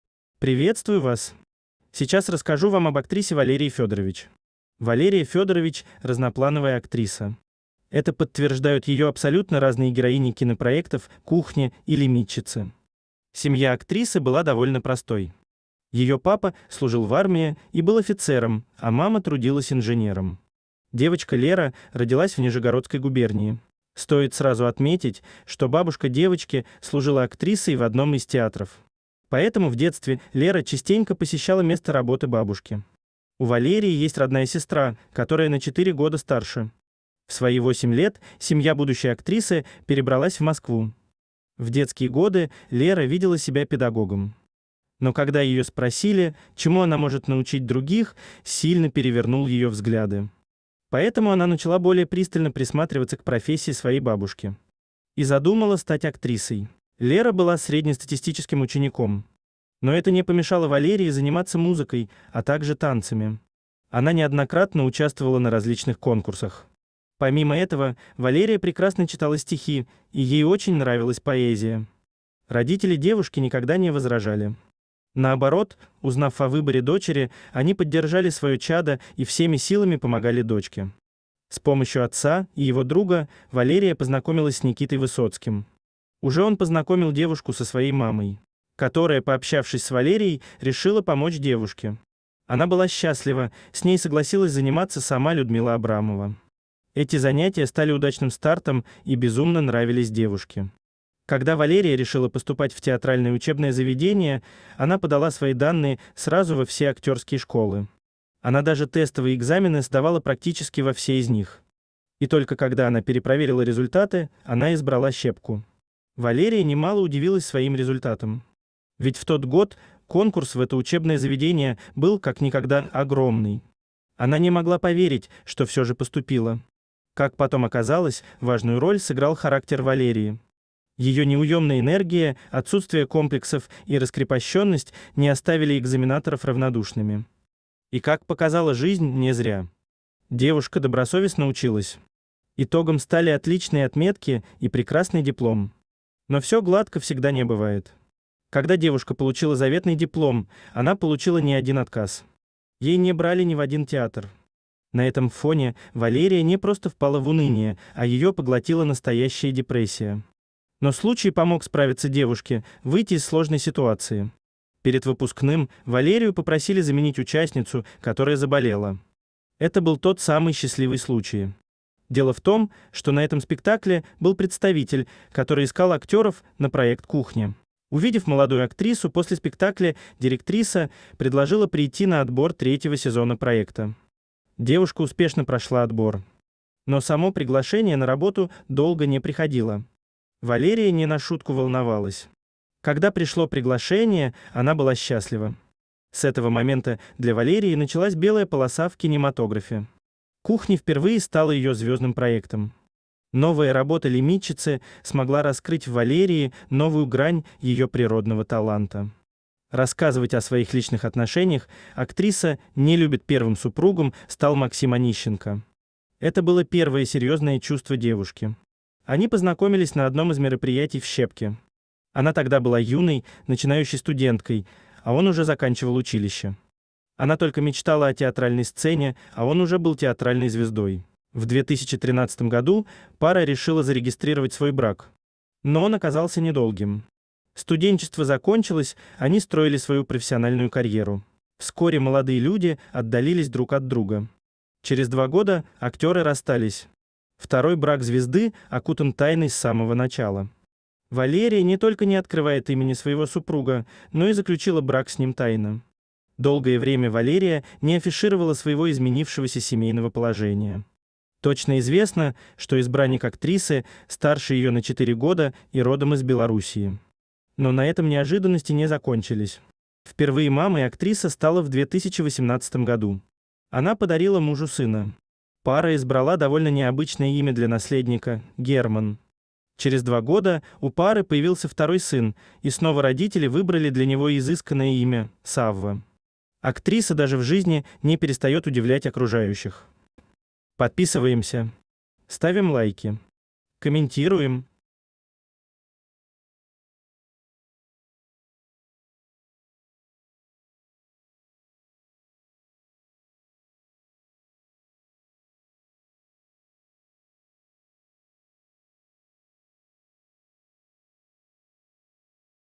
Валерия Федорович. Развелась с известным актером, тайно вышла замуж, 2 сына с редкими именами.
Семья актрисы была довольно простой. Её папа- служил в армии и был офицером, а мама трудилась инженером. Девочка Лера родилась в Нижегородской губернии.